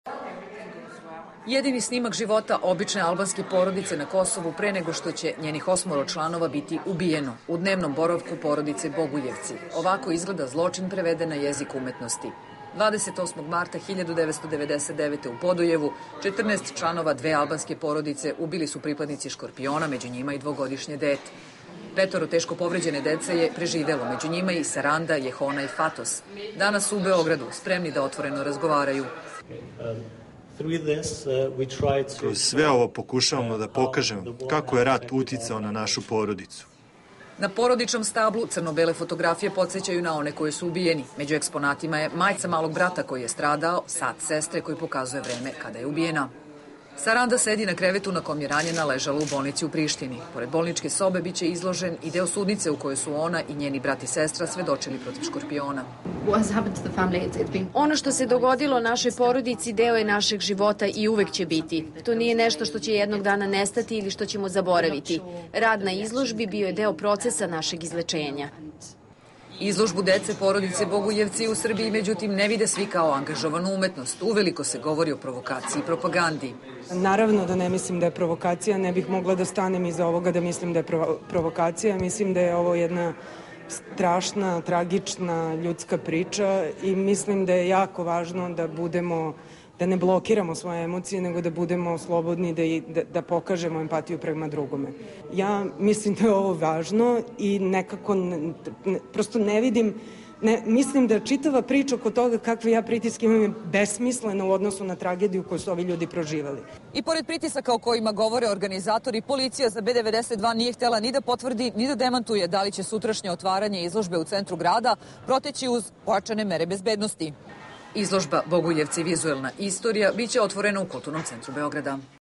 Oprečene reakcije o izložbi Bogujevci/Vizuelna istorija
Izložba "Bogujevci / Vizuelna istorija", najavljena kao "omaž svim porodicama i žrtvama", još nije otvorena, a već izaziva oprečne reakcije. Dok je izložba, koja se bavi zločinom koji su 1999. počinili pripadnici "Škorpiona" nad članovima porodice Bogujevci, za jedne poziv na razgovor, za druge je provokacija.